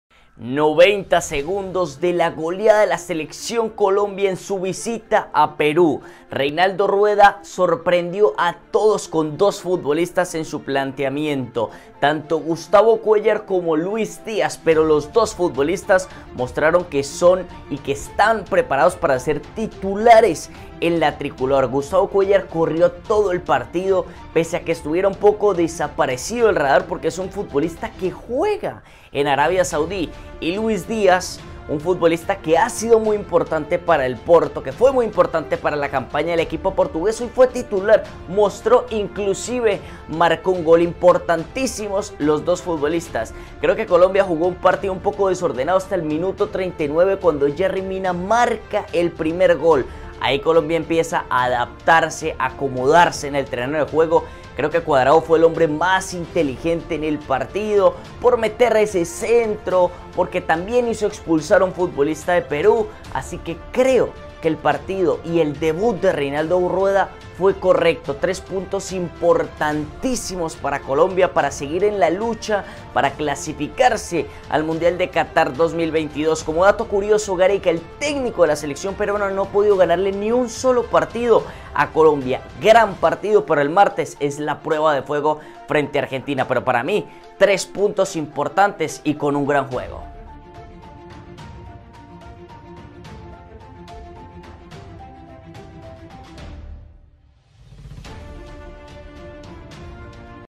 MI COMENTARIO DE LA VICTORIA DE COLOMBIA FRENTE A PERÚ
Colombia salió victorioso en su visita a Peru. El planteamiento táctico de Reinaldo Rueda fue positivo para golear y mostrar un buen juego. Lastima la expulsión de Daniel Muñoz, no pudimos ver el gran juego que mostró en el Genk. 3 puntos que meten a Colombia en la lucha para clasificar a Qatar 2022

VIDEO HECHO Y EDITADO POR: Juan Fresquet Ceballos

J3 Media:
Canal de Deportes (J3 DEPORTES)